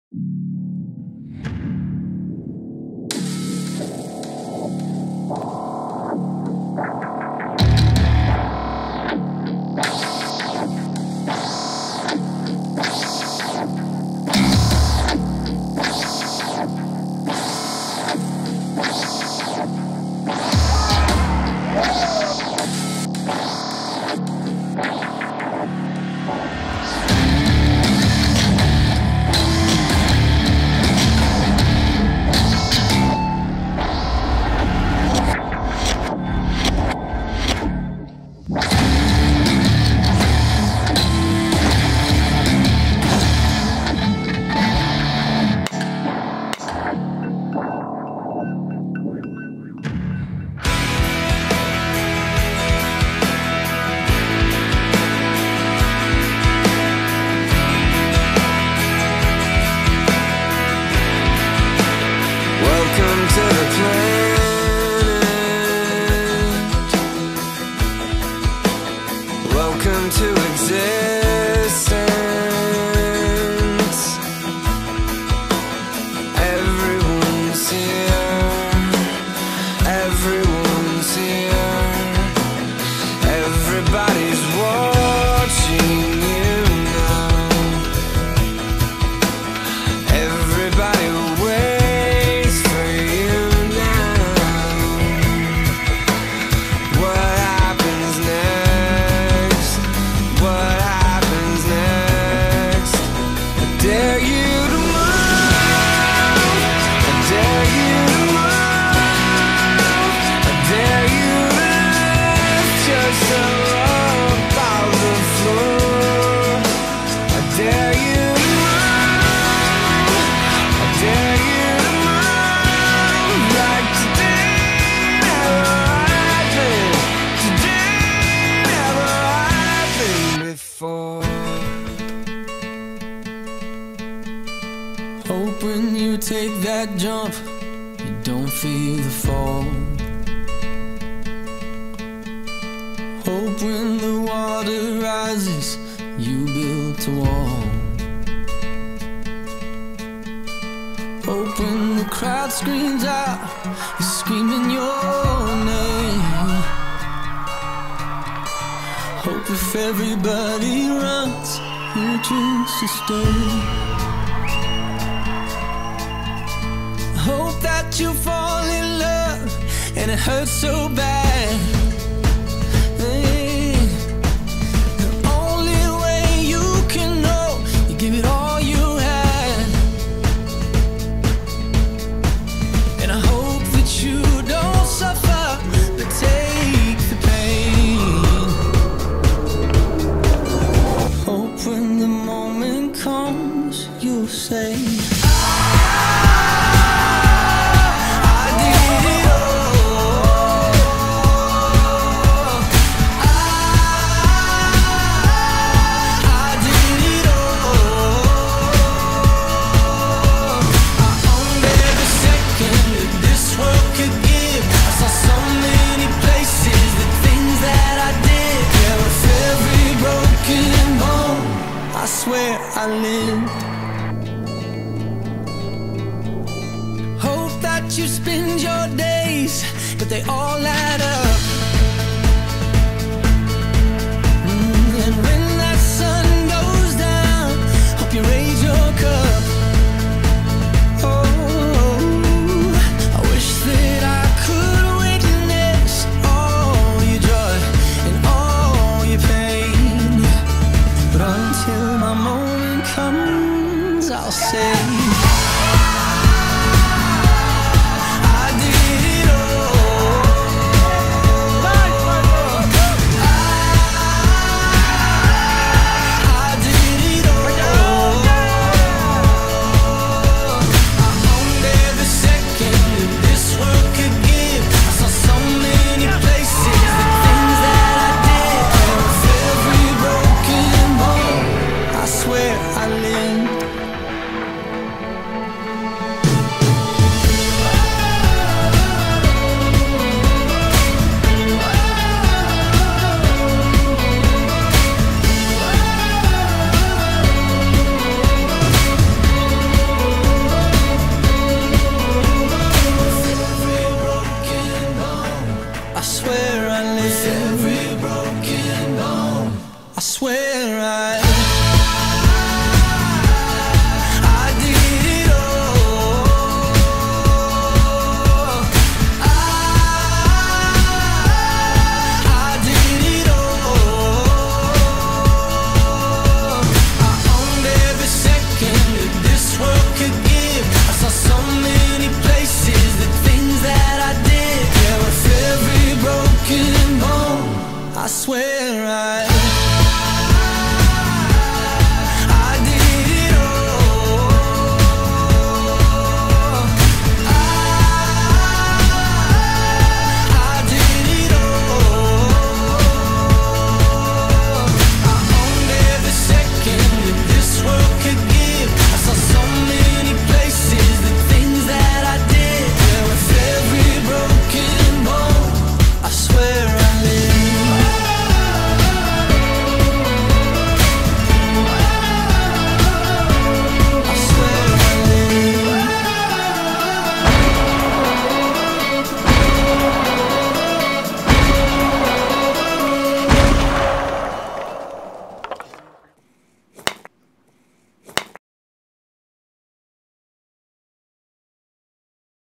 Boulder Hockey Club Banquet Season Video 2017 - 2018
The end of another Season.  Great kids great times!